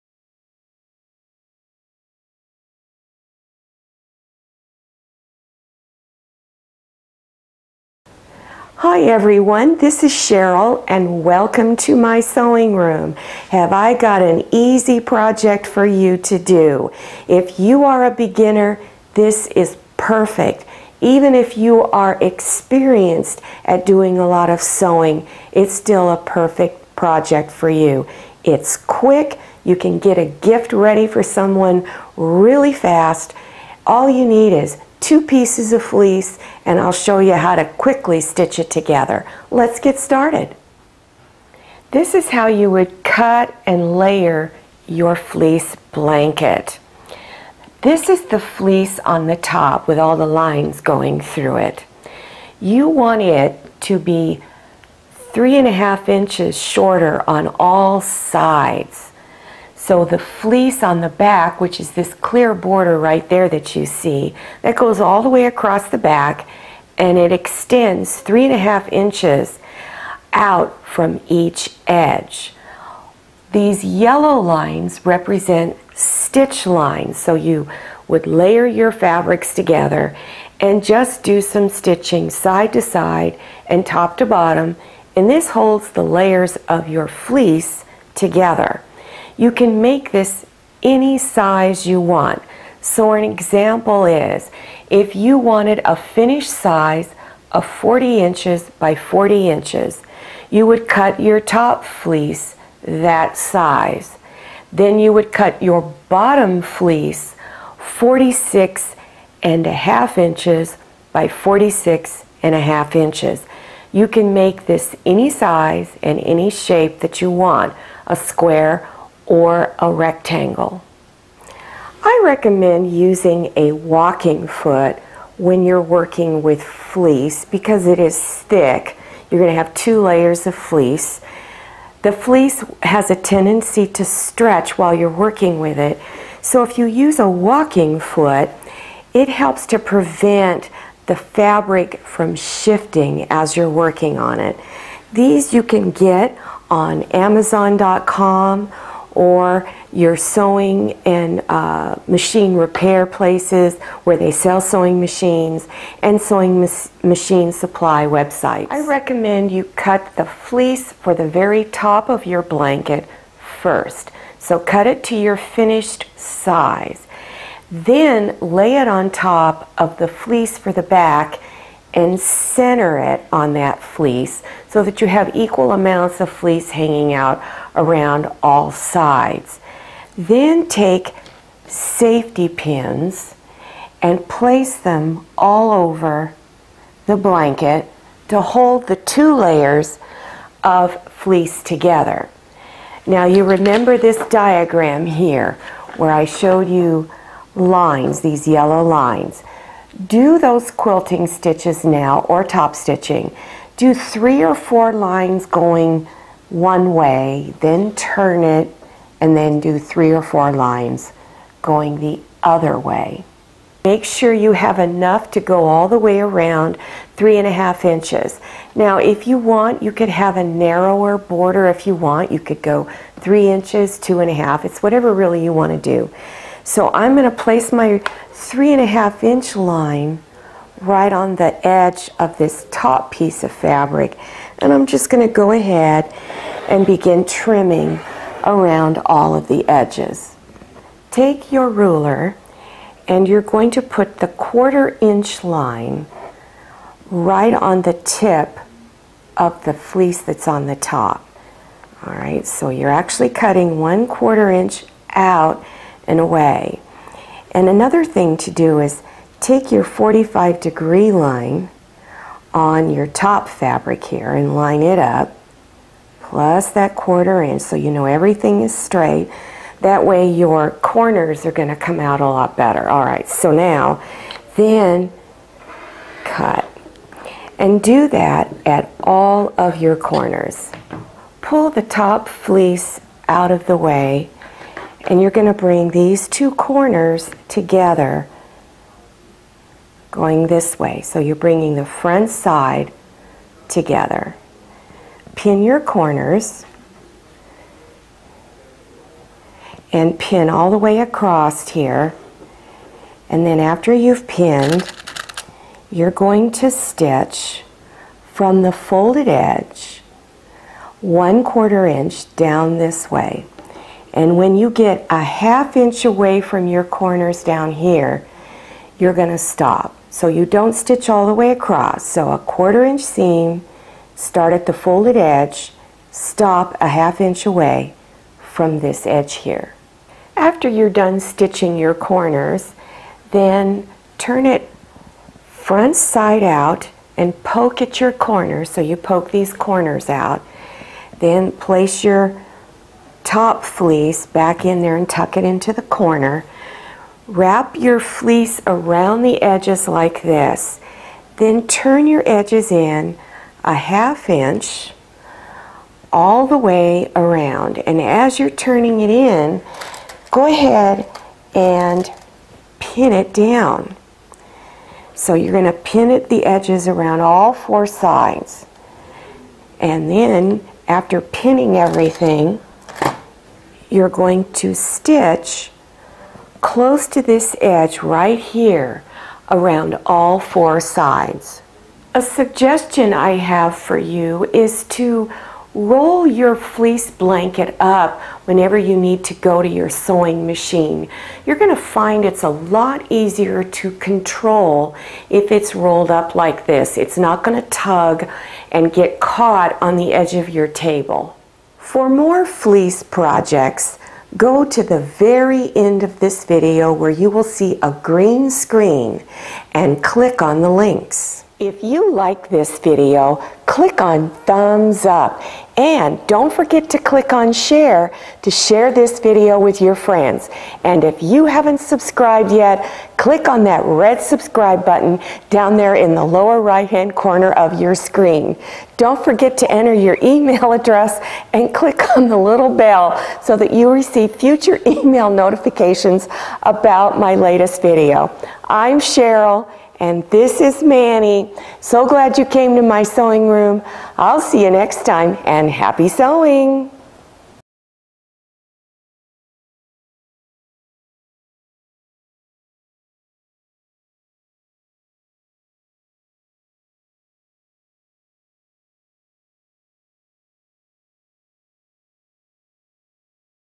Beginners Fleece Blanket
Beginners Fleece Blanket:  use two different fleece fabrics, very quick to make, very easy to make, very detailed instructions.